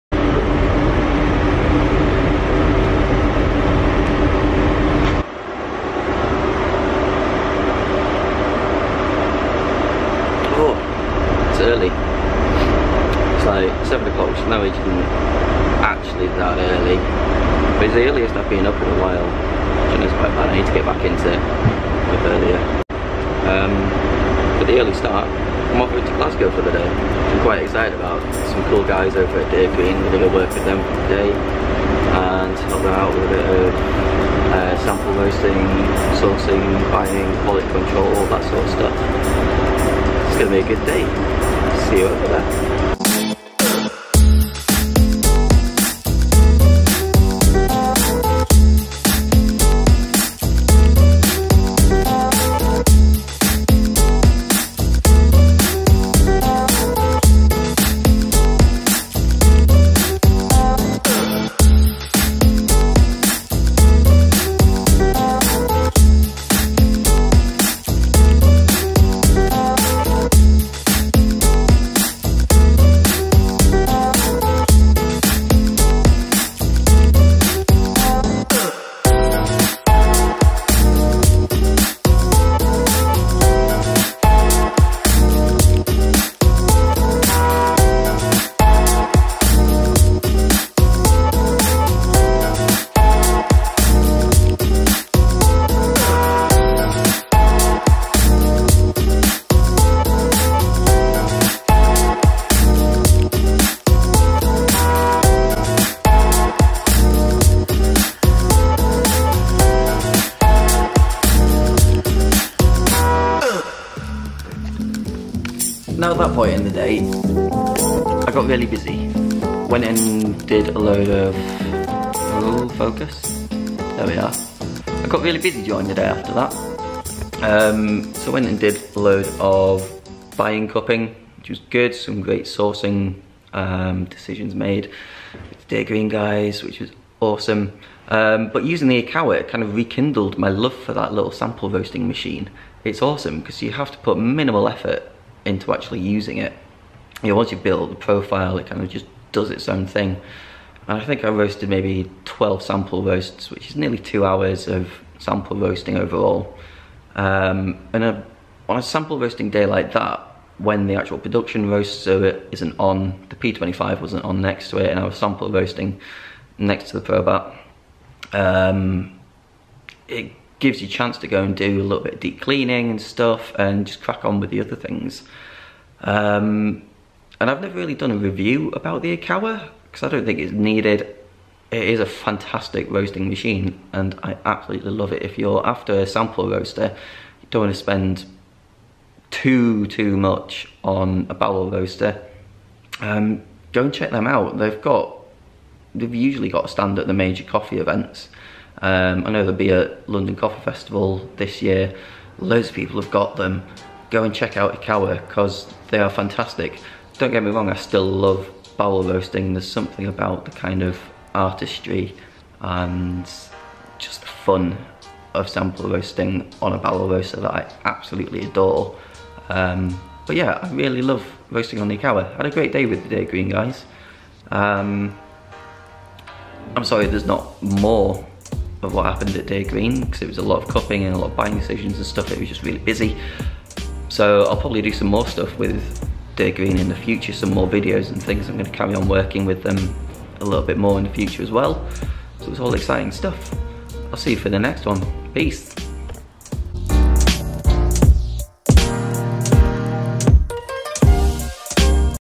My Love affair with Ikawa
I was working in Glasgow for the day with the lovely Dear Green folks, and got back to using the Ikawa sample roaster, its been a while and I had forgotten how much I enjoy this little machine!

Sorry the end is so out of focus!!!